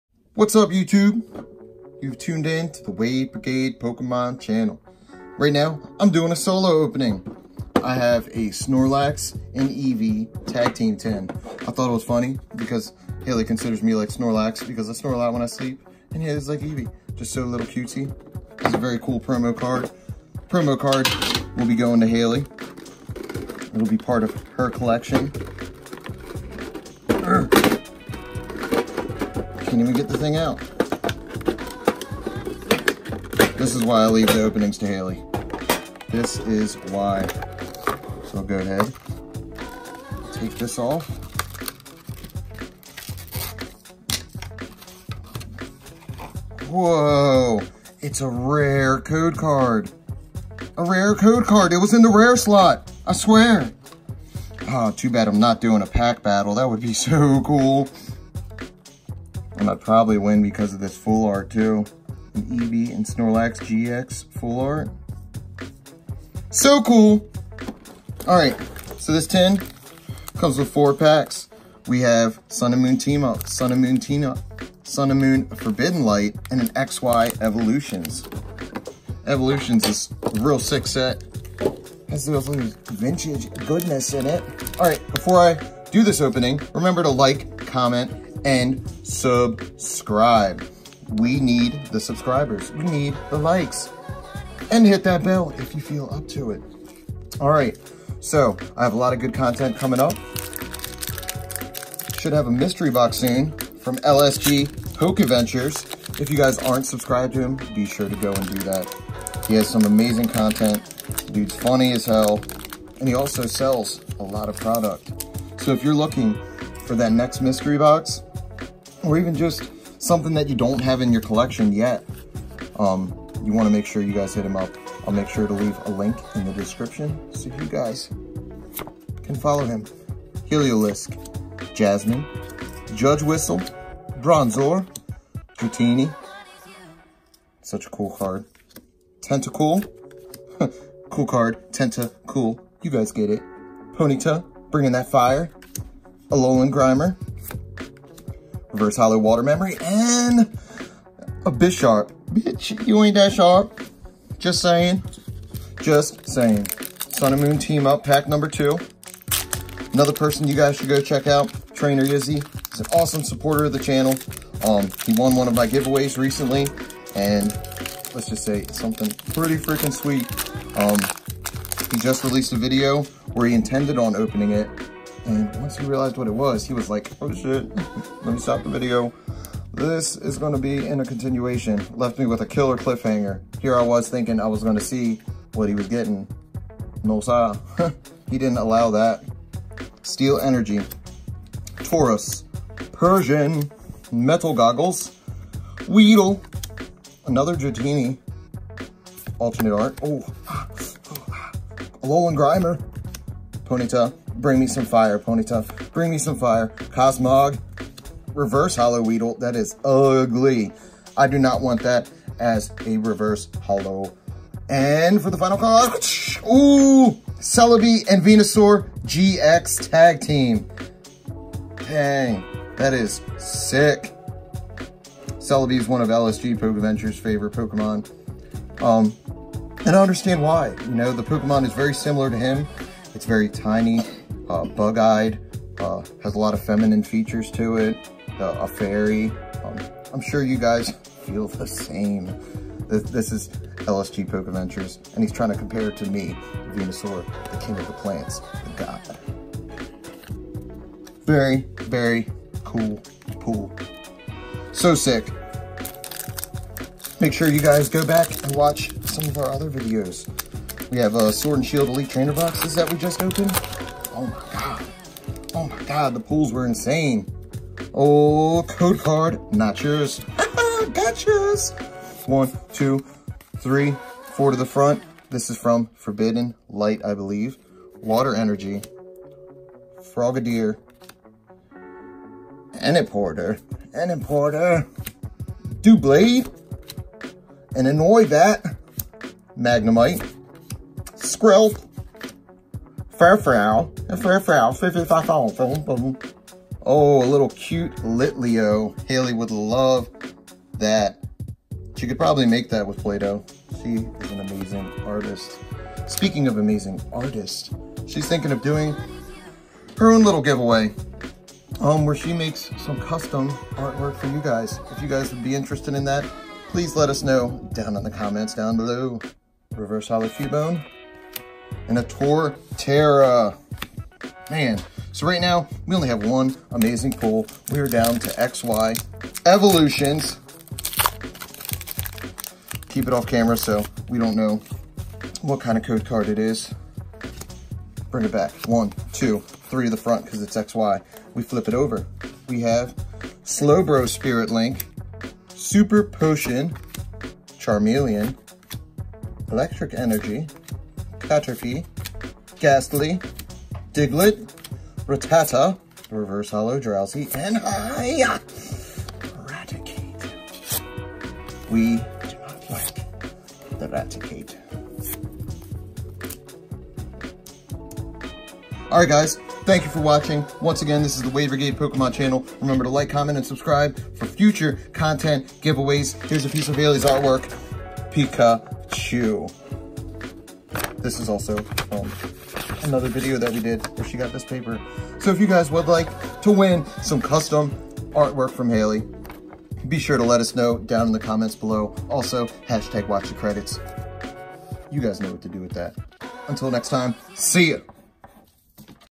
*THIS POKÉMON OPENING MADE ME STUPID!!*
Opening the perfect tag team tin. Snorlax and eevee are like me and haileigh. Definitely gives us a laugh. Check out who I pulled as well. Be sure to show some love to LSG Pokeventures too.